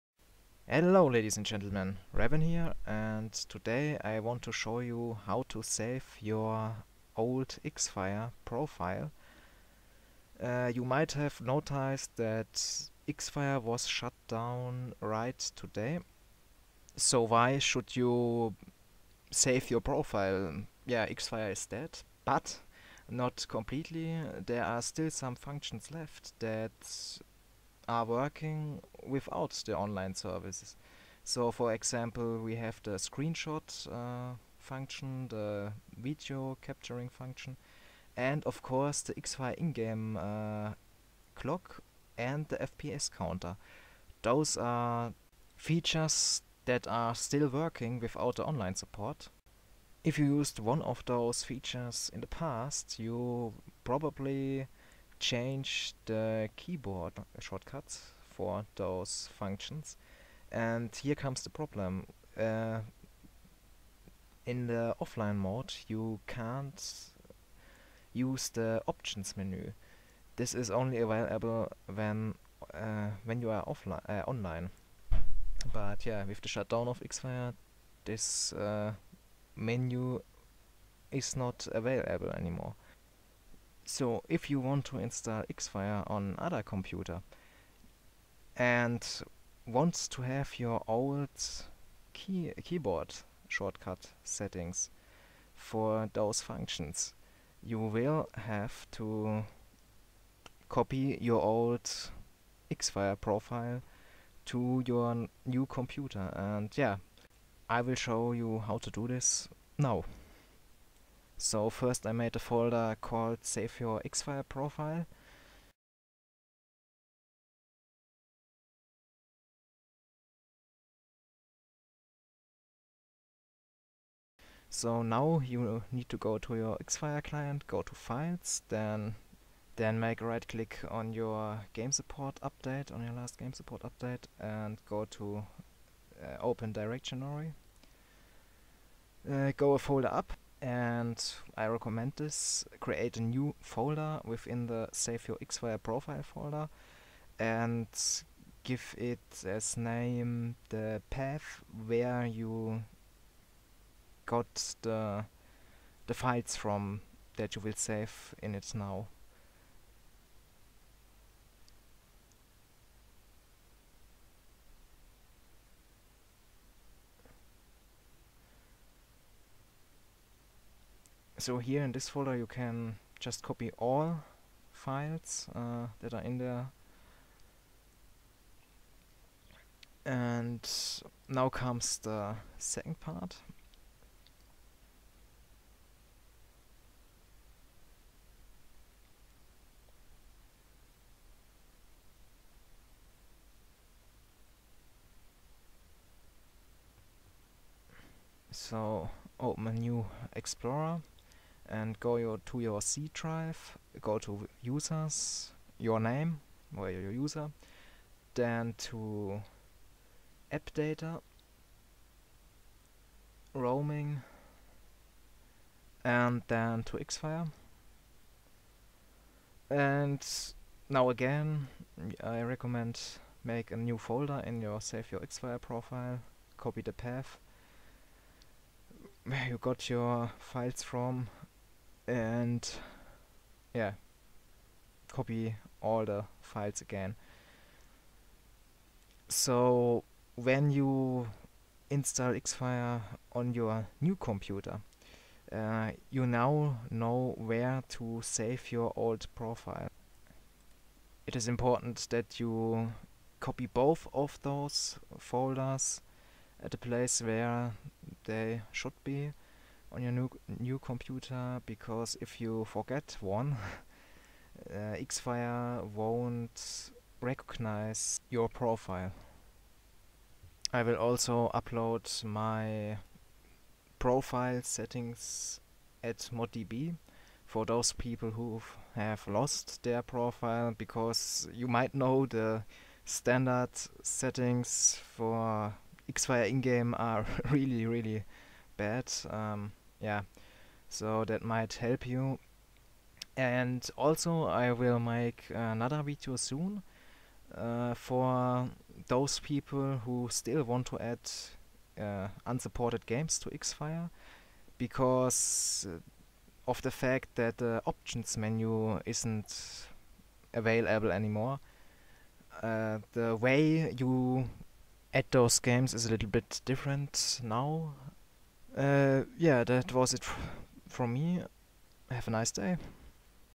Xfire shutdown - How to save your profile settings (not your screenshots or your videos)
This video shows how to save your profile settings for Xfire. Your profile is needed if you install Xfire on another computer and want to keep your keyboard shortcut settings.

If you lost your profile and want to use Xfire with accaptable settings for ingame shortcuts, you can download my profile settings here: